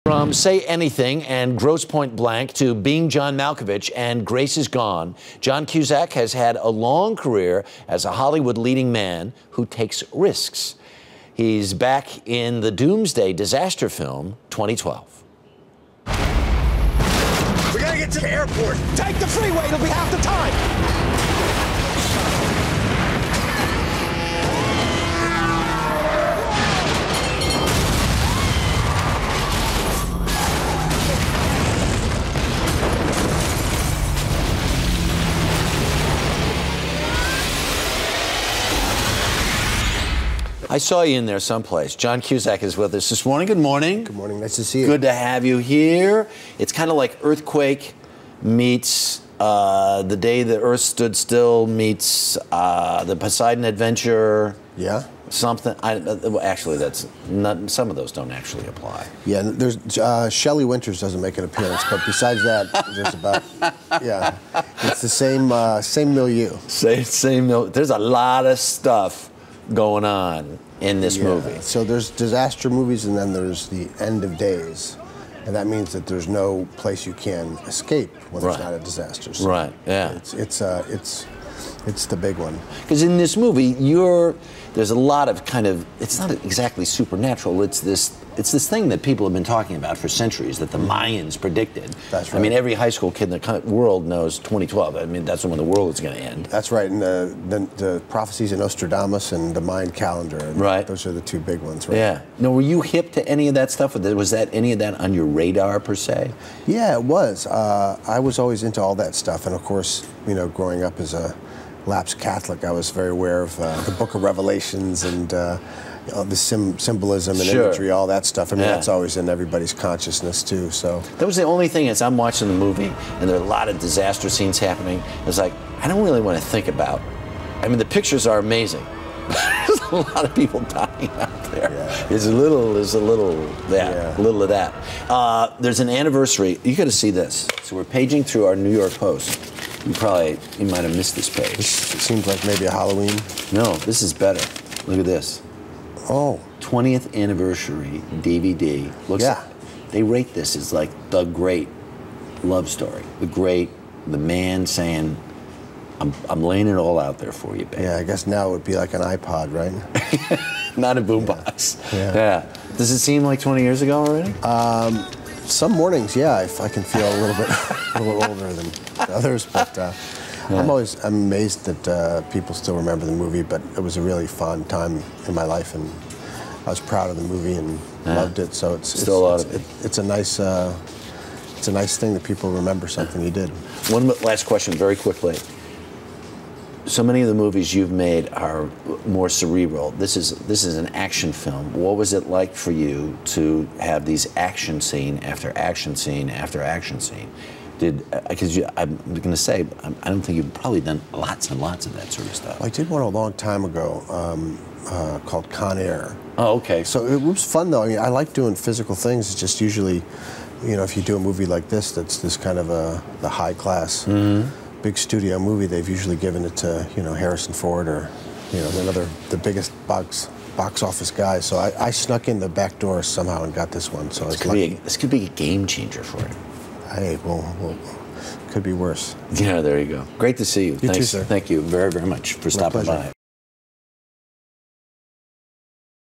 John Cusack's 2012 Blockbuster
Harry Smith spoke with actor John Cusack about his new action role in the disaster film, 2012.